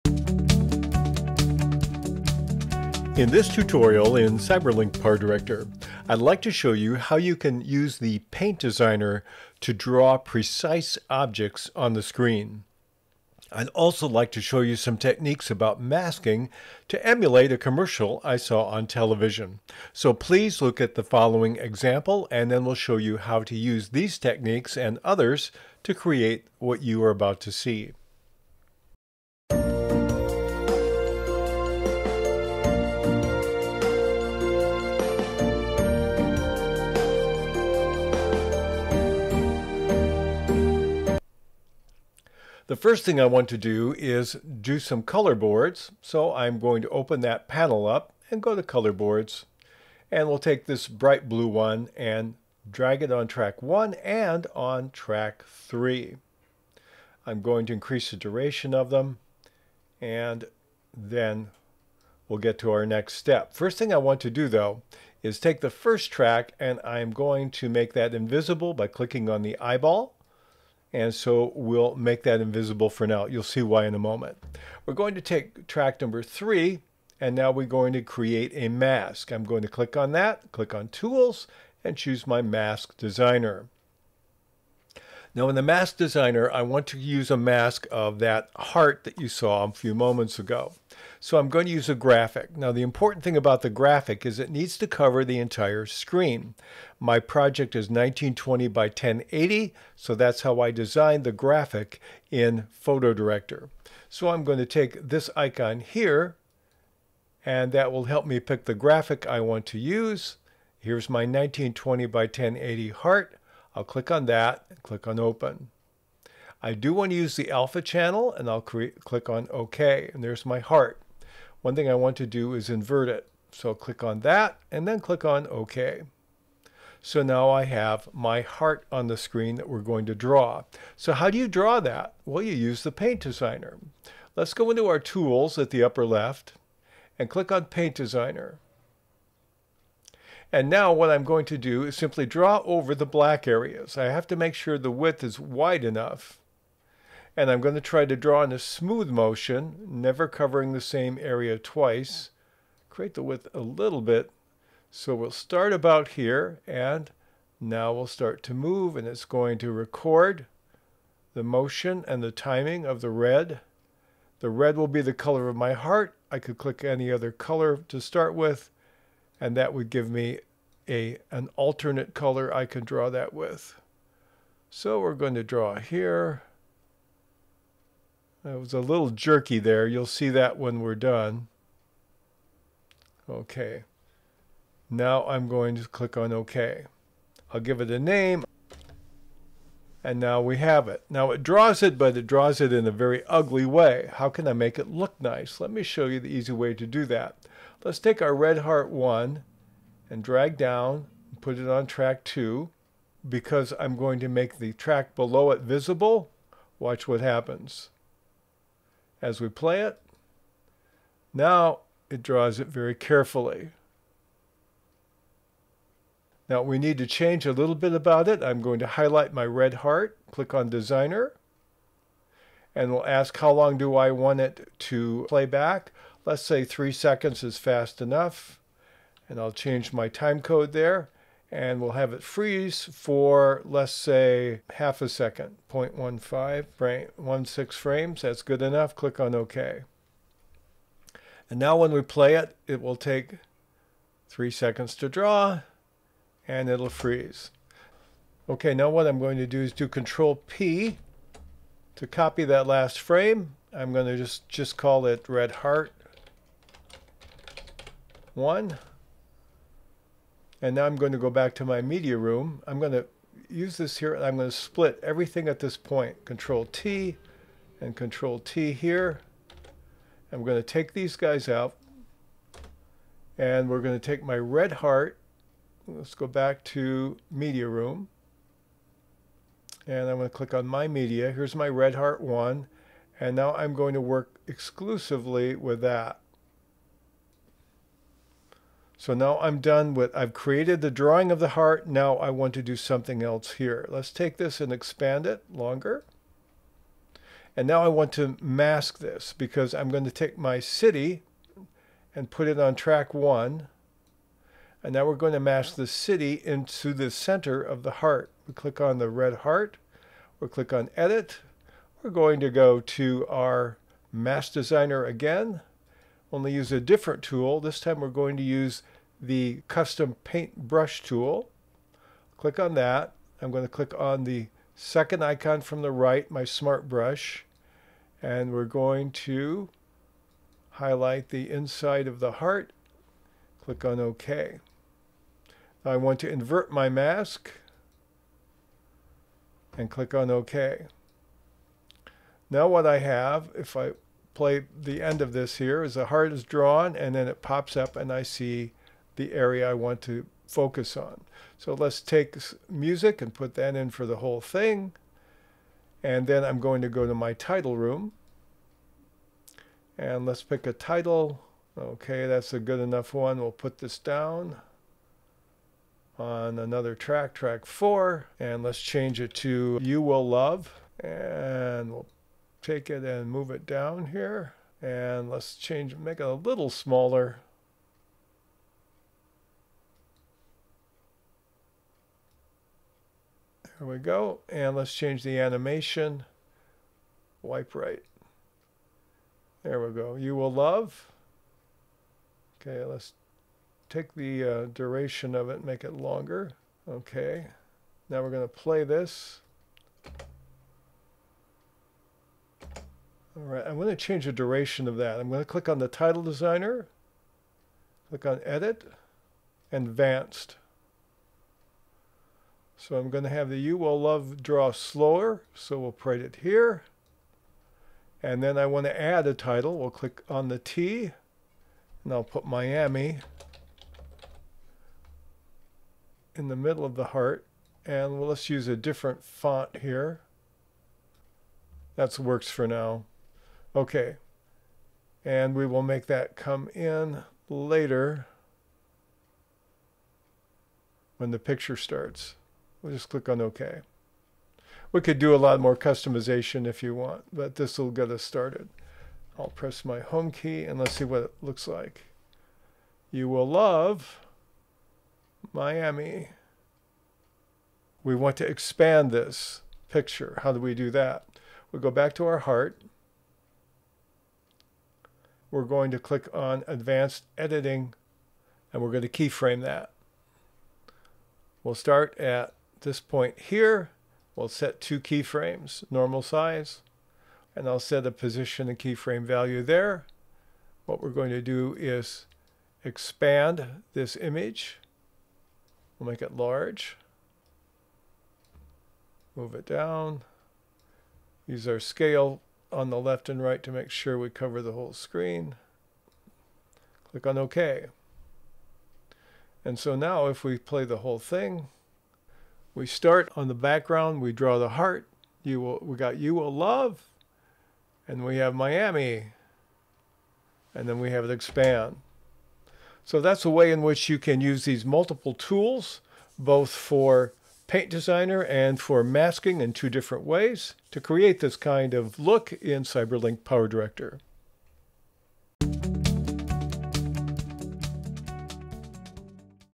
How to draw precise objects with paint designer in PowerDirector
Paint designer allows you to record animated drawing and place it on the screen. But if your hand moves the paintbrush awkwardly, it will show. We show you how to make it look like you are drawing an object, but while actually tracing an object that you already have available. And we add some tips related to masking and achieving a different reveal effect as well.